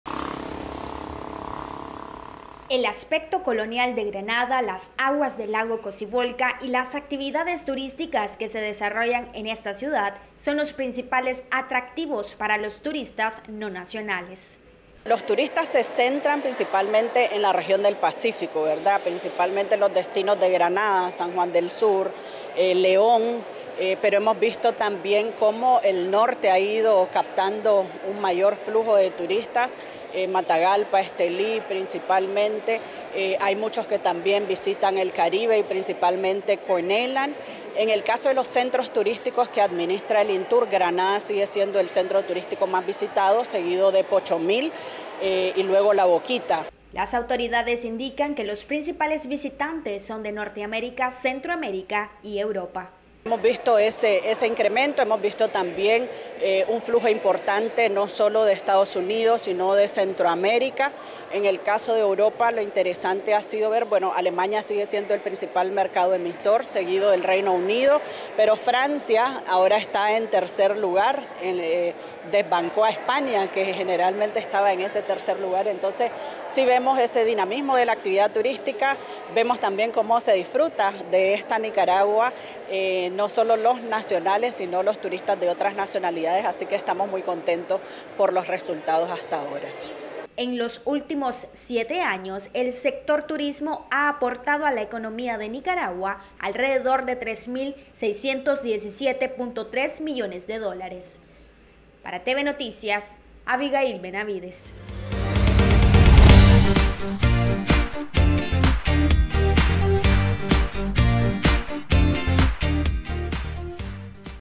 Granada, la ciudad más visitada por extranjeros
El destino más visitado por los turistas es Granada, debido a su aspecto colonial y las diversas actividades que se pueden realizar en esta bella ciudad a sus 500 años de fundación, así lo manifestó la codirectora del Instituto Nicaragüense de Turismo.